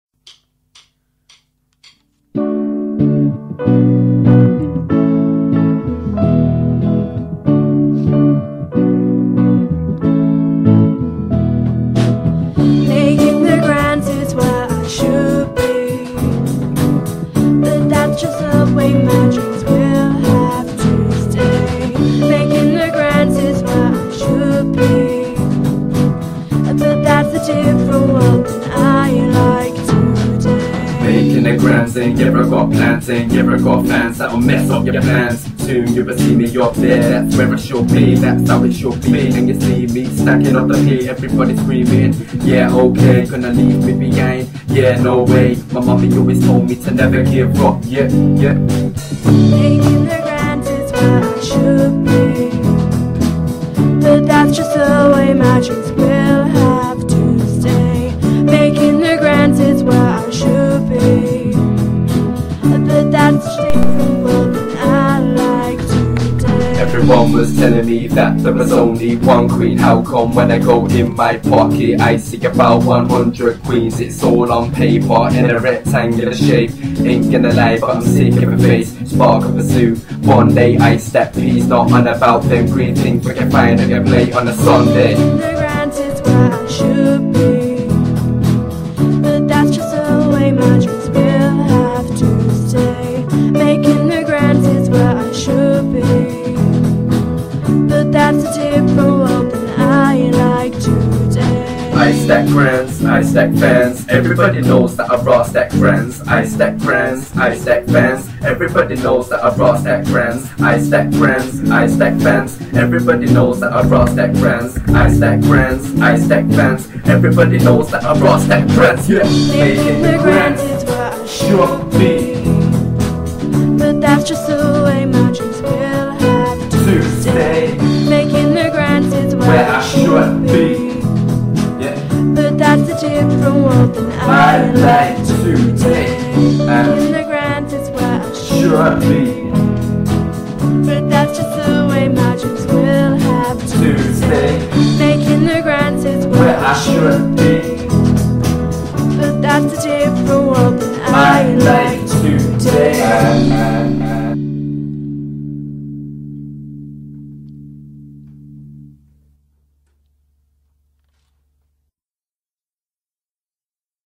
Jake's Unit 2 Composition
Thanks to:
Tom / Scoonii - Rapper
Anna - Singer
Will - Bass
Sam - Guitar
Georgia - Pianist